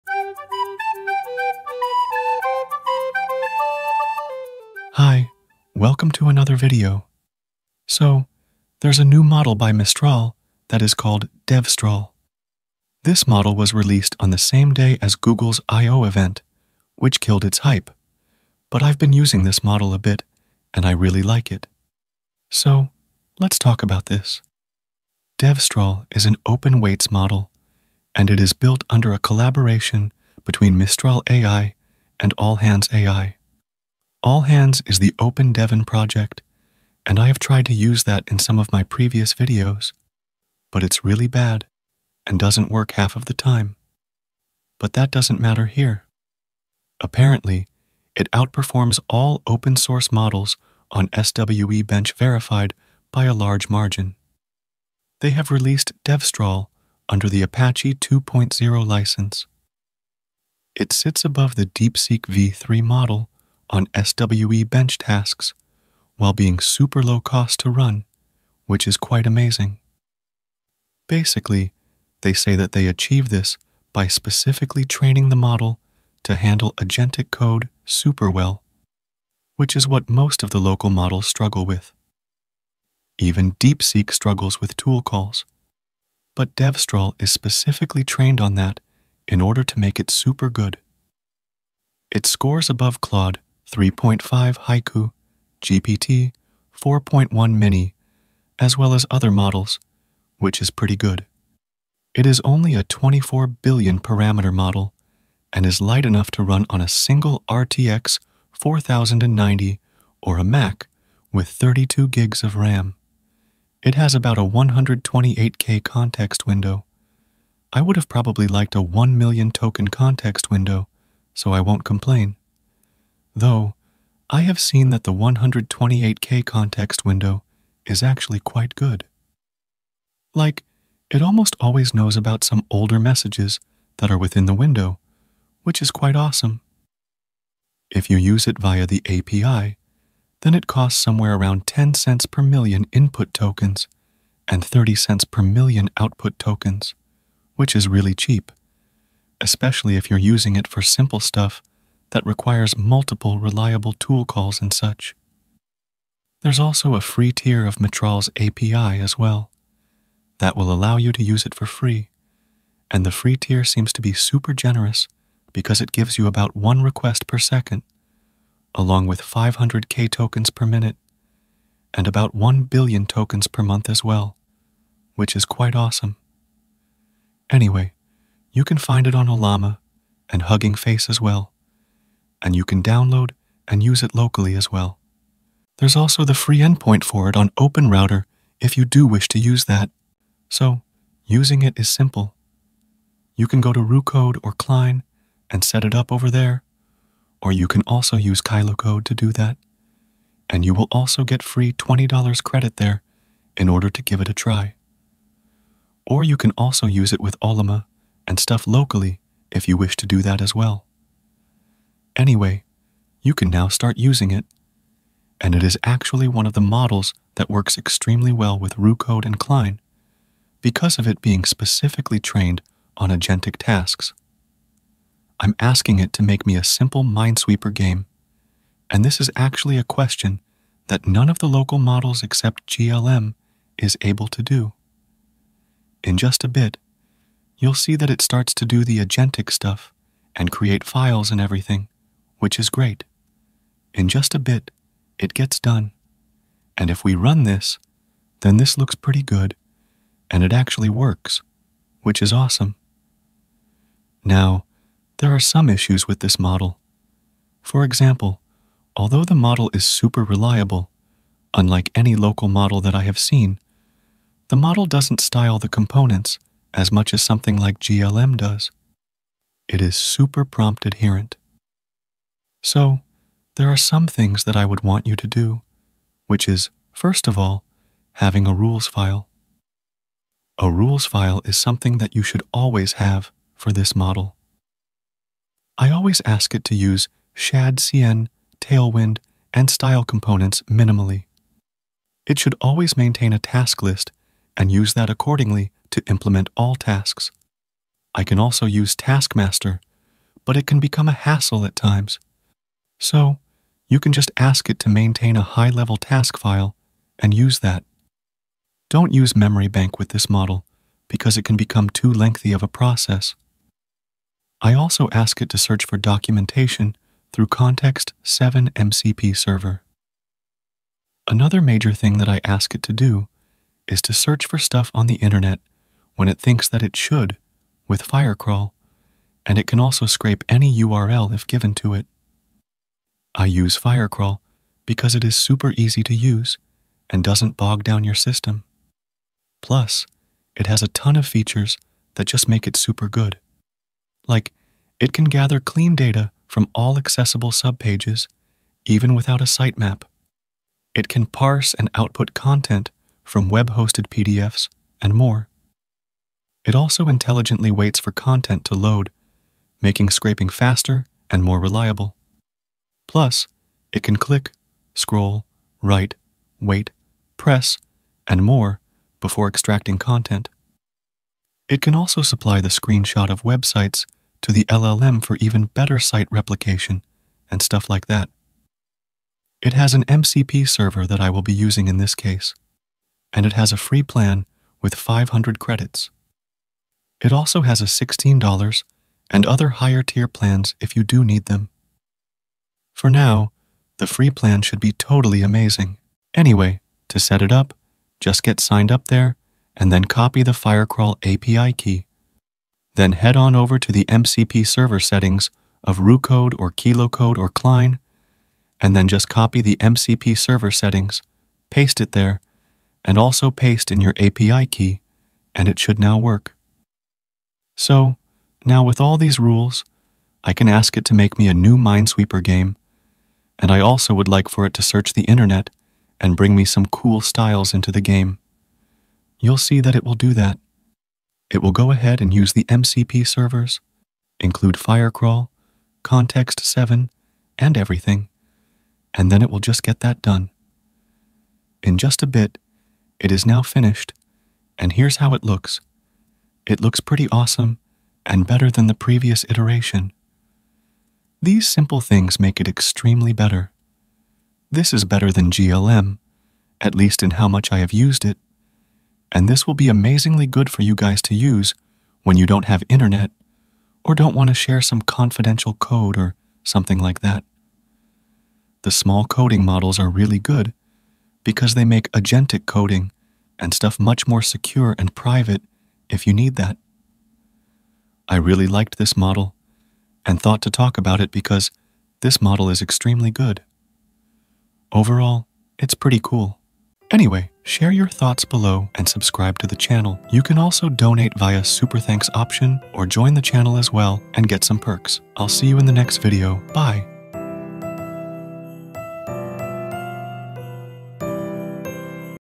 Devstral + FREE MCPs + Unlimited FREE API: This is LOCAL SONNET! The best LOCAL CODER COMBINATION!
In this video, I'll be telling you that how you can use the new local and private Mistral's Devstral with Free MCPs, Unlimited Free API and create an alternative to sonnet locally.

Key Takeaways:

🚀 Devstral is a new open-weights AI model from Mistral, built in collaboration with All Hands AI.
💡 It outperforms all open-source models on SWE-Bench Verified tasks by a large margin.
💸 The model is cost-effective to run, with low API pricing and a generous free tier.
🖥️ Devstral can run locally on a single RTX 4090 or a Mac with 32GB RAM, making it accessible.
🧠 It handles agentic coding tasks exceptionally well, especially tool calls and task management.
🌐 Integration with tools like Firecrawl and MCP Server enables advanced web search and scraping.
👍 Overall, Devstral is reliable, prompt-adherent, and a strong choice for secure, private coding.

Timestamps:

00:00 - Introduction
00:12 - Devstral Overview
03:10 - Usage with RooCode, Cline, Kilo
05:20 - Firecrawl Integration
06:53 - Usage